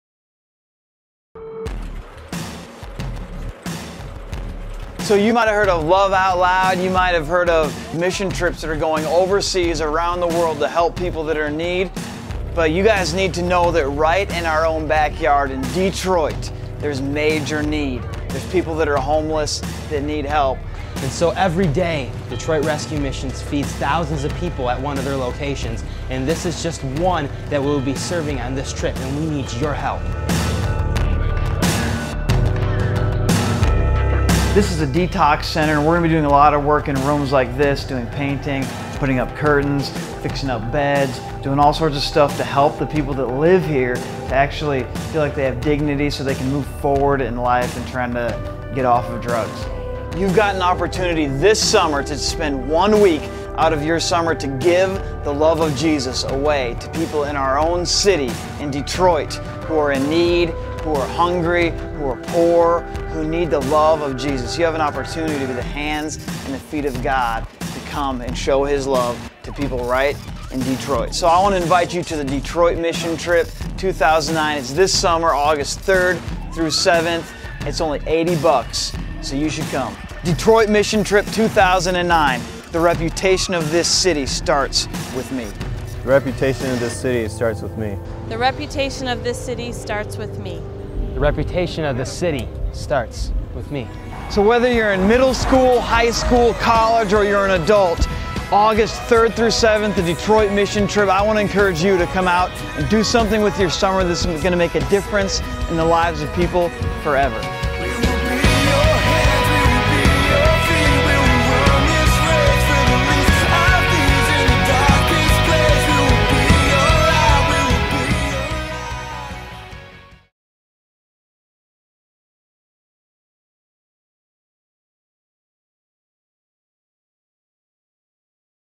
Detroit Mission Trip 2009 Promo
on August 3-7th, 2009 metro south church youth ministries will head to the City of Detroit to give the Love of Jesus to the people in need!  You should join us!